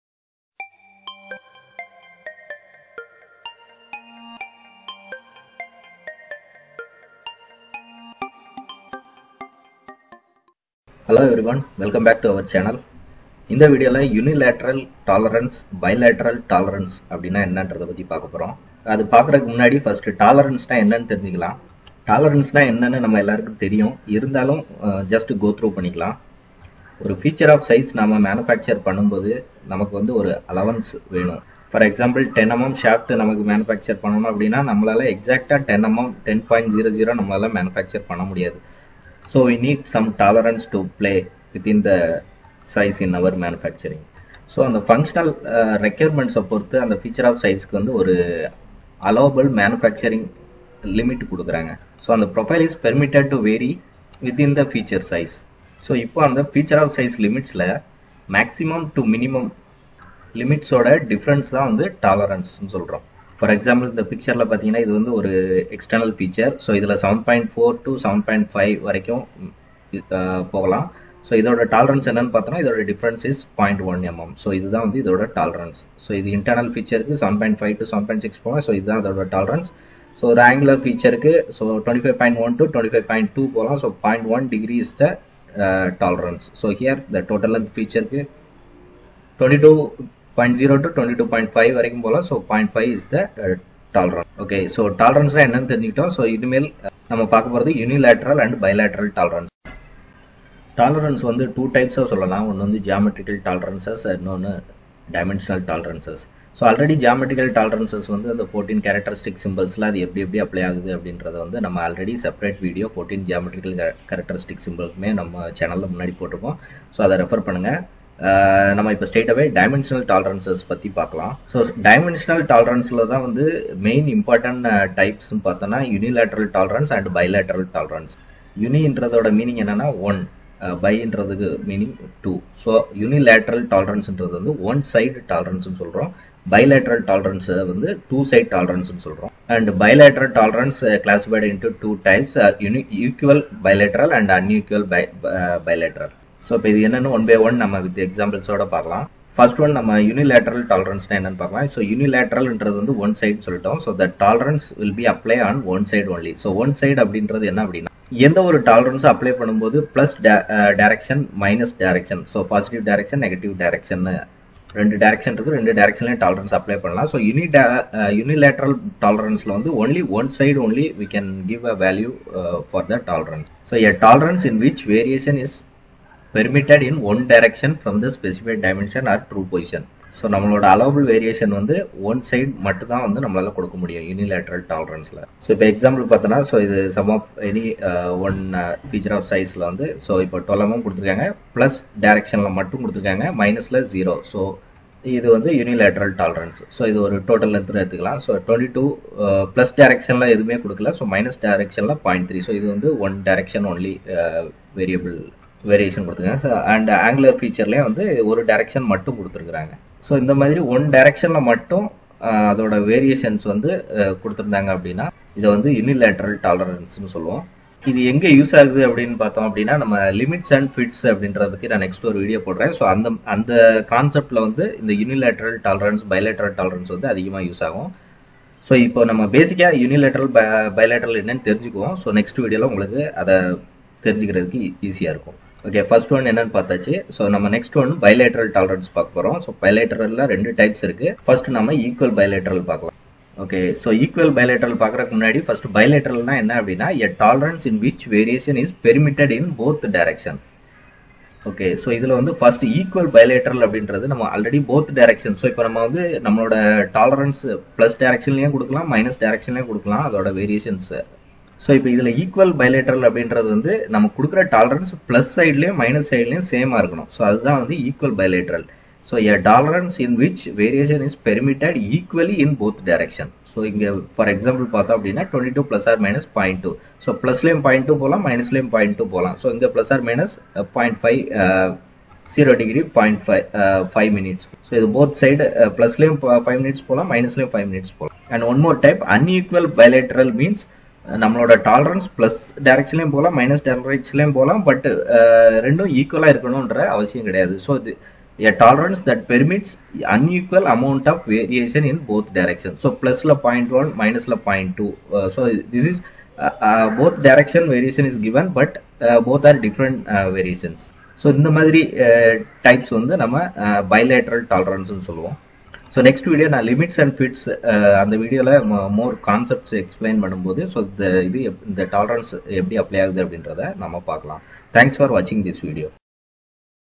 What is Tolerance | Unilateral and bilateral tolerance explained in tamil | Fits and tolerances
This video is discussed about the below topics in tamil
1) what is tolerance
2) Types of tolerance
3)  Unilateral and Bilateral tolerance